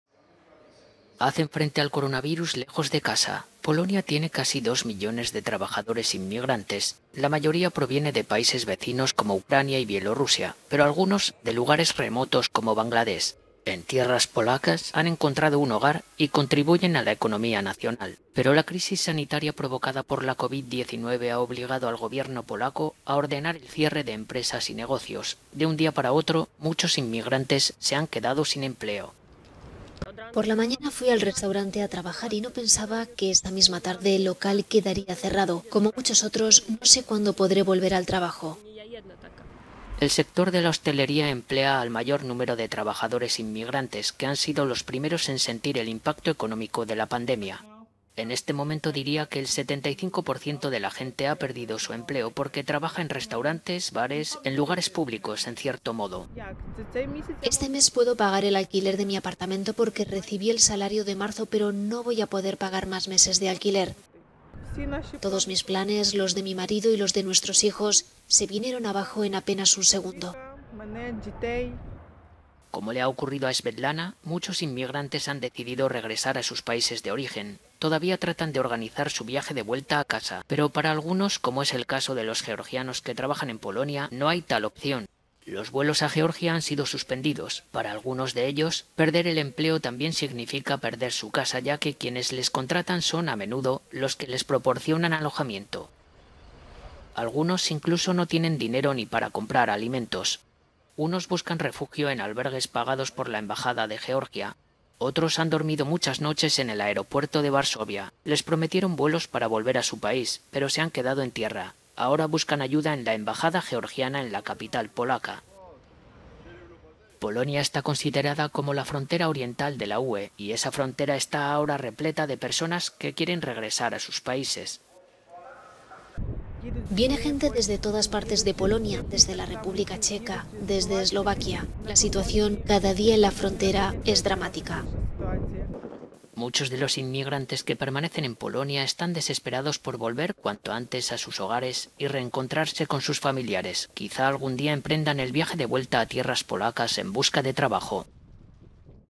El drama laboral para los inmigrantes provocado por el coronavirus
Polonia tiene casi dos millones de trabajadores inmigrantes. La mayoría proviene de países vecinos como Ucrania y Bielorrusia pero algunos, de lugares remotos como Bangladés. En tierras polacas han encontrado un hogar y contribuyen a la economía nacional.…